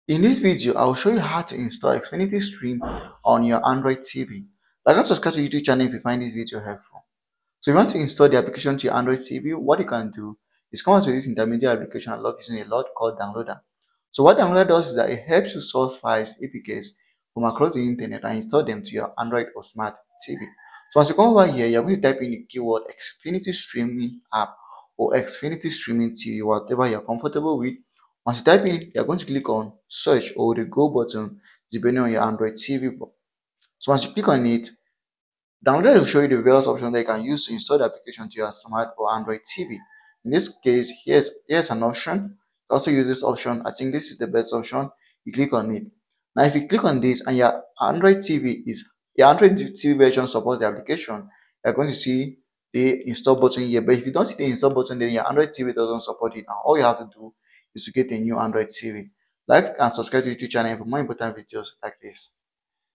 How to Install Xfinity Stream App on ANY Android TV in 2025 (Without Google Play Store)
In this tutorial, I will show you How to Install Xfinity Stream on Android TV 2024. If you have been searching for it, in this video, you will learn How to Install Xfinity Stream on Android TV 2024 without google play store on your smart or android tv.